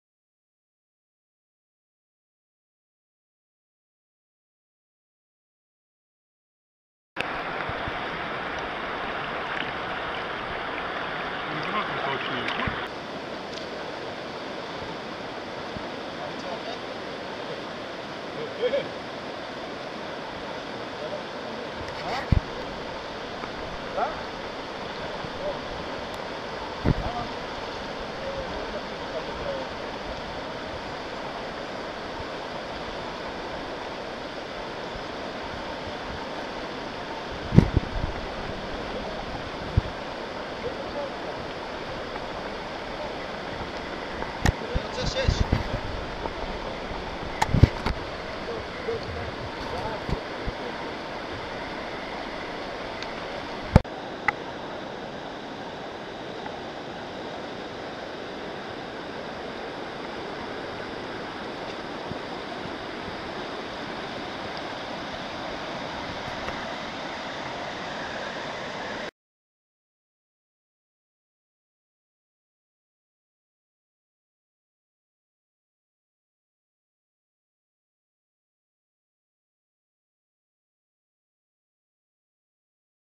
Yarden River Spinning
Fishing in the beautiful Yarden river in Israel.Splendid nature sights unnatural to Israel. Great fishing and good catch and release.